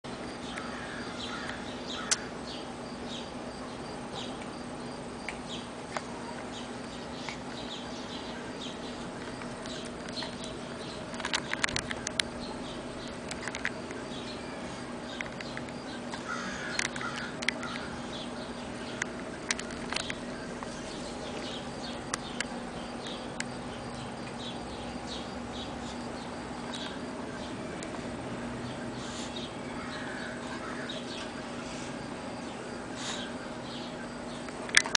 SID THE TOY POODLE FETCH THE BALL
SID DOING WHAT HE DOES BEST AND MAYBE TRYING TO SPEAK